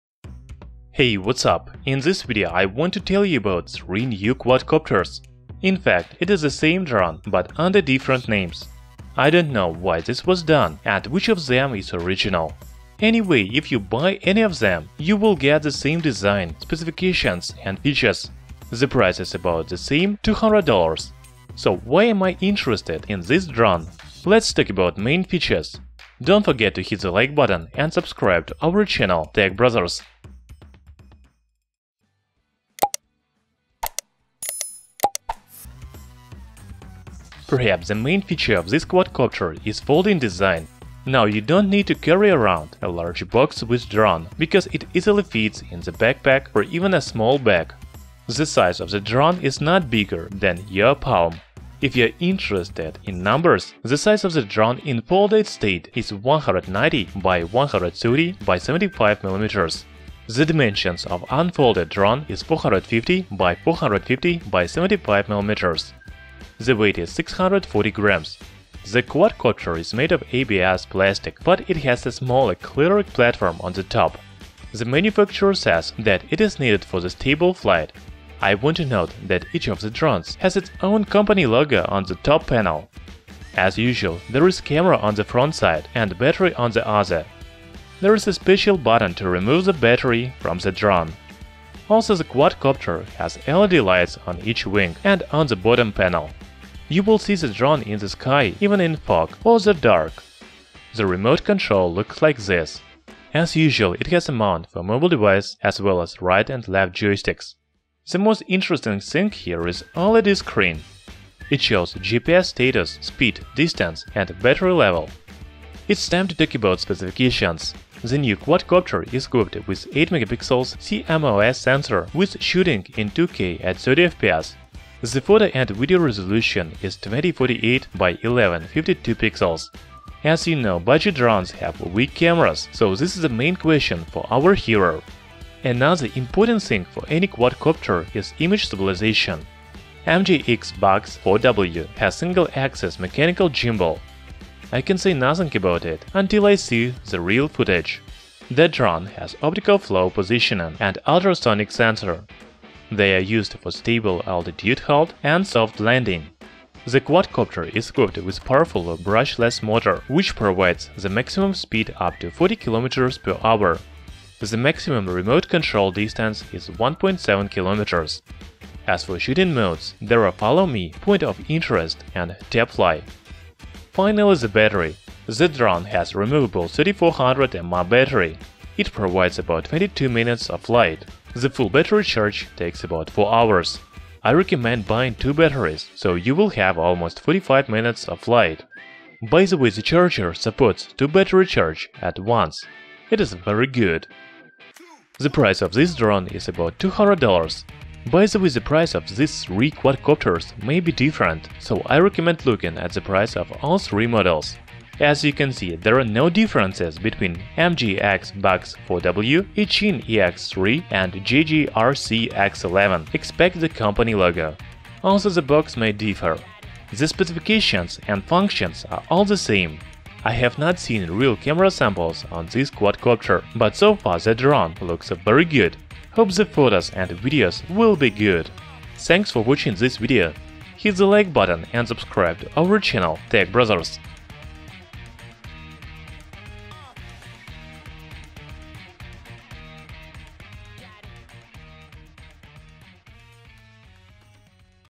MJX BUGS 4W, Eachine EX3 and JJRC X11: The Same Drone Under Different Names
Watch our video about MJX BUGS 4W, Eachine EX3 and JJRC X11. These RC drones have the same design, specifications and features, but but they are released under different names and from different brands. - GearBest

Aircraft & Remote Controller:

Dimensions Unfolded: 450×450×75mm
Folded: 190×130×75mm
Weight (with Battery) about 640g
Color Black
Diagonal Size 330mm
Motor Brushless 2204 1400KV
ESC 8A
Max Flying Time above 20mins
Built-in Positioning Sensor GPS / Optical Flow / Barometer / Ultrasonic
Hovering Accuracy (Indoor) Vertical: ± 0.3 m
Horizontal: ± 0.3 m
Hovering Accuracy (Ondoor) Vertical: ± 0.5 m
Horizontal: ± 1.5 m
Max Ascending Speed 3m/s
Max Descending Speed 2m/s
Max Flying Speed 40km/h
Flight Altitude Limit 150m
Max Control Distance above 1.6KM
Wind Resistance Rating
Operating Frequency 2.4G
Remote Control Battery AA*2 (NOT Included)
Usage Time About 12h
EIRP 20dbm

Camera & Gimbal :

Camera 2K 5G WiFi
APP X-DRONE
Photo Resolution 2048*1152
Video Resolution 2048*1152
Local Video Frame Rate 20
FPV Frequency 5G WIFI
FPV Distance 300m-400m
Lens (Viewing Angle) 110°
Aperture f/2.4
Focal Length 1m to ∞
Sensor 1/3" CMOS
Effective Pixels 2M
ISO Range Video / Photo: 100-1600 (Automatic)
Shutter Speed 1/30s-1/10000s
File System FAT32
Extended Memory Maximum support 32GB
Memory Card Micro SD-Card Class10 and above
Gimbal Type Single Axis
Pitch Rotation Range 0°-90°

Battery:

Type Li-Po
Voltage 7.6V
Capacity 3400mAh
Weight 165g

Features:
- With Foldable and portable design, the drone is easy to carry.
- With 5G wifi function can be connected APP, APK system to take pictures, video, real-time transmission through the phone camera image
- Full HD 2K camera. Support shooting 2K 30fps with 8MP CMOS sensor,wide-angle lens,retain the most original details,make your creation more inspirational.
- EX3 equipped with single-axis mechanical gimbal,which can reduce shake and keep shots stable effectively.
- Camera angle can be remote controlled to pitch from -90° to 0°.
- Ultrasonic sensor assists barometer to keep super stable altitude hold.
The EX3 camera will lock on your mobile phone,when using this function, the aircraft will track you automatically and capture your movement with a particular aerial view.
- Point of Interest. Set a specific building,object or position as your point of interest.With point of interest,your aircraft will continuously circle clockwise around the preset point.
- Optical Flow Positioning. The built-in optical flow sensor that gets the real-time optical flow information and makes integrating computations.
- Tap Fly. In the waypoint flight mode,you can just focus on composing,EX3 will fly to the target or fly in the direction you tapped on the screen,and change the flight paths smoothly if you re-planed the track on the screen again.
- Smart remote control alarm function.The transmitter buzzer receives the voltage status of the drone will send out beep sound once the drone is in low volatage.
- OLED screen. Assists the drone to fly in any flight environment.
- Left / Right throttle control switch mode.
- The charging progress will displayed on the charger, and can be charged 2PCS battery at the same time.(Innovative use of multi-head input, users can choose to use one input line when charging two batteries (the total charging time is about 8 hours), or you can use two input lines to connect two USB adapters, the fastest 4 hours full.)

- With 7.6V 3400mAh battery, the fly time up to 20 mins.
- With 4 colorful LED lights assist night flight.
- The quadcopter fuselage is made of high strength and resistant engineering plastics,lightweight and durable resistance.

Function: Forward/Backward, Turn left/right, Up/down, 3-level flight speed, 2K Camera, WiFi FPV, GPS, Optical Flow Positioning, Altitude Hold Mode, Follow Me, Point of Interest, Tap Fly, Surrounding Flight, LED Light

Hashtags for searching